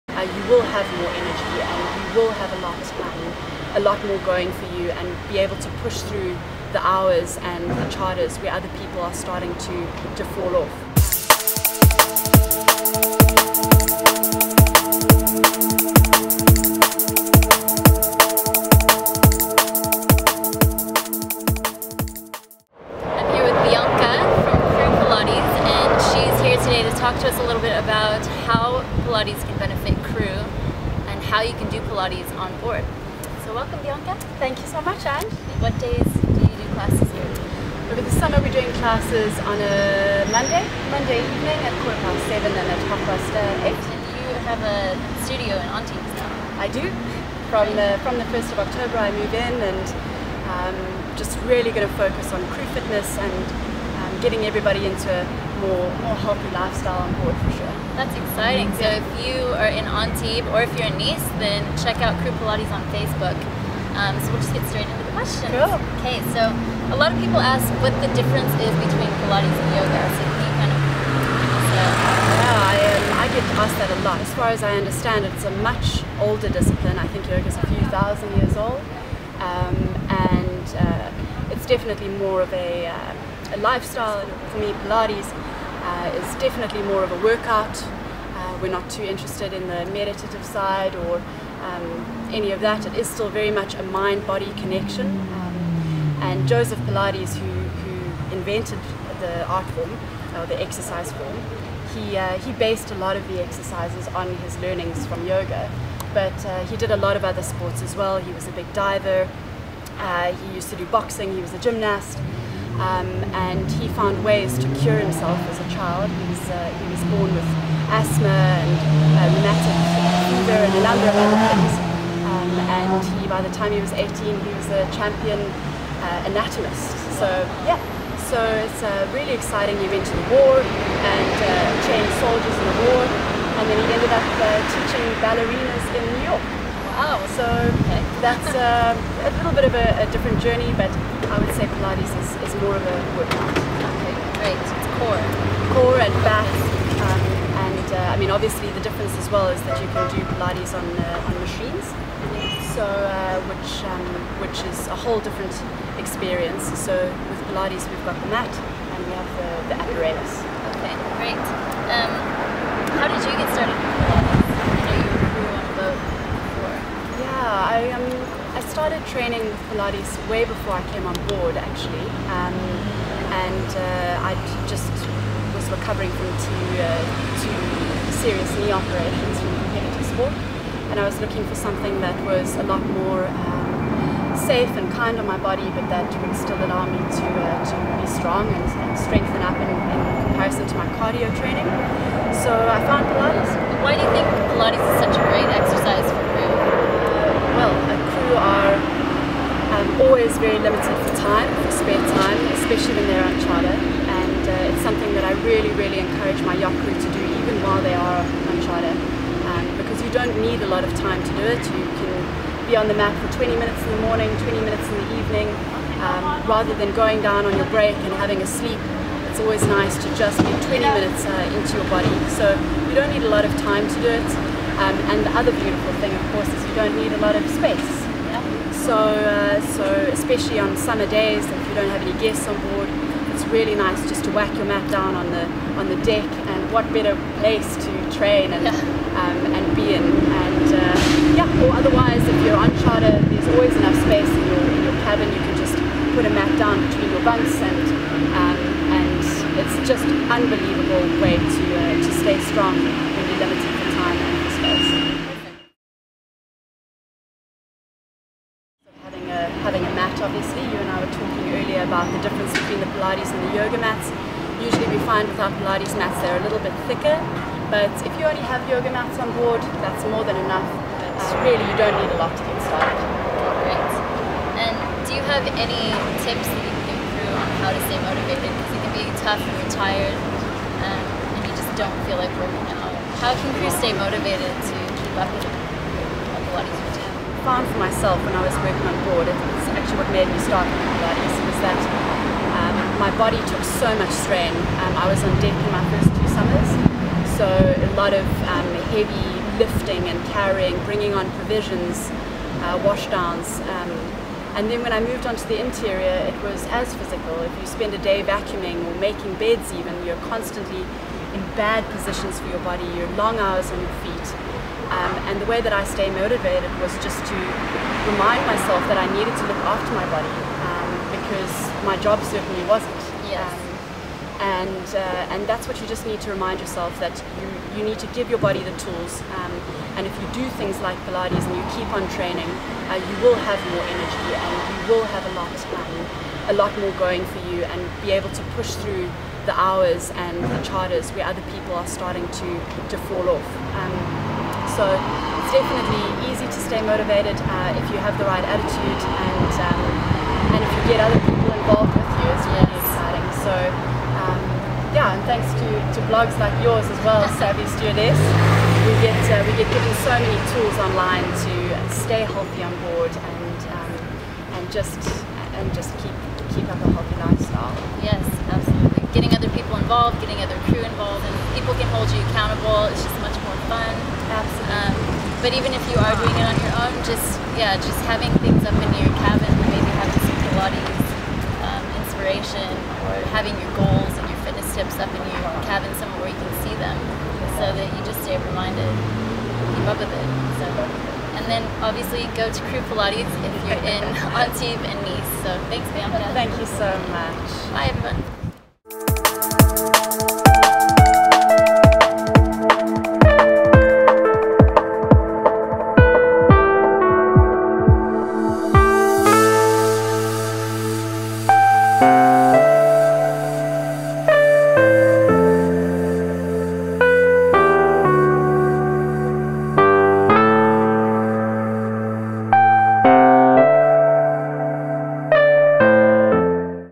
Why You Should Be Doing Pilates on Board
Bianca of Crew Pilates explains how Yacht Crew can get fit and strong on board with Pilates.

Studio Address: 20 Place Audiberti, Antibes 06600

Download a free copy of The Beginners Guide to Working on a Yacht Ebook